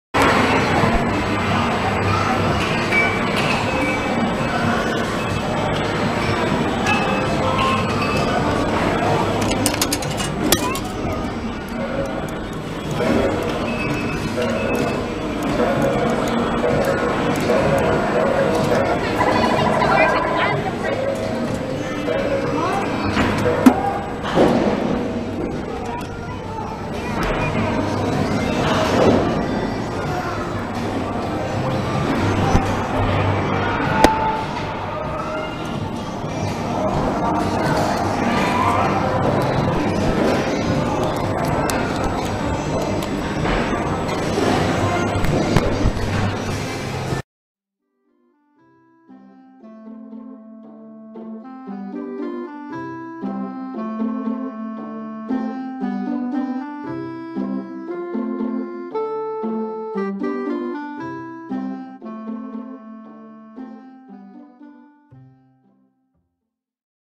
Coin Operated Find a Key Crane Amusement Arcade Machine
Filmed at Wellington Pier, Great Yarmouth on June 9th 2021.

If anybody has any further information about this machine, I would love to hear from you.